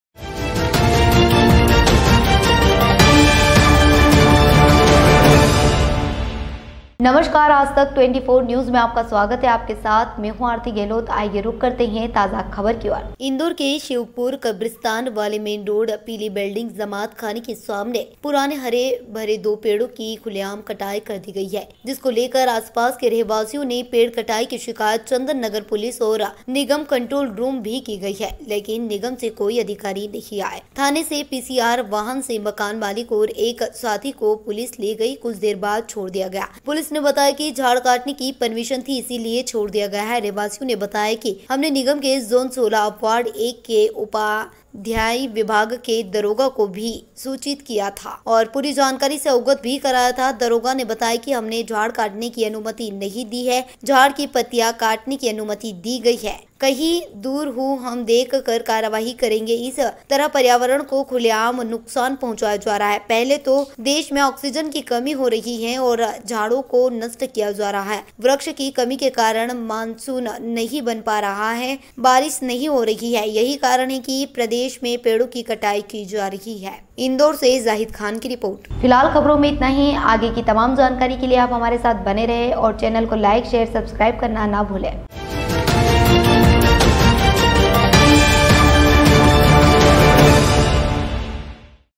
खुलेआम हरे भरे पेड़ों की कटाई पर अंकुश लगाने पर नाकाम न्दौर के शिरपुर कब्रस्तान वाले मेन रोड पील
खुलेआम हरे भरे पेड़ों की कटाई पर अंकुश लगाने पर नाकाम
इन्दौर के शिरपुर कब्रस्तान वाले मेन रोड पीली बिल्डिंग जमात खाने के सामने पुराने हरे भरे दो पेड़ों की खुलेआम कटाई कर दी गई ' आस पास के रहवासियों ने पेड़ कटाई की शिकायत चंदन नगर पुलिस और निगम कंट्रोल रूम भी की गई मगर निगम से कोई अधिकारी नही आये थाने से पी सी आर वहाँन से मकान मालिक और एक साथी को पुलिस ले गई 'कूछ देर बाद छोड़ दिया गया ' पुलिस ने बताया की झाड़़ काटने की परमीशन थी .इसीलिये छोड़ दिया गया ' रहवासियों ने बताया की हमने निगम के   झौन 16/वार्ड एक के उध्यान विभाग के   दरोगा  को भी सूचित किया और पूरी जानकारी से अवगत भी कराया गया ' दरोगा ने बताया की हमने झाड़ काटने की अनुमति नही दी हॆ
झाड की पत्तियाँ काटने की अनुमति दी गई हॆ ' मॆ कही दुर हूँ हम देख कर कार्यवाही करेंगे ' इस तरह पर्यावरण को खुलेआम नूख्सान पहुँचाया जा रहा हॆ ' पहले तो देश मॆ ऑक्सीजन की कमी हो रही हॆ ' और झाडो को नष्ट किया जा रहा हॆ ' वॄक्ष की कमी के कारण मान सुन नही बन रहा हॆ बारिश नही हो रही हॆ यही कारण हॆ प्रदेश मॆ पेड़ों की कटाई की जा रही हॆ .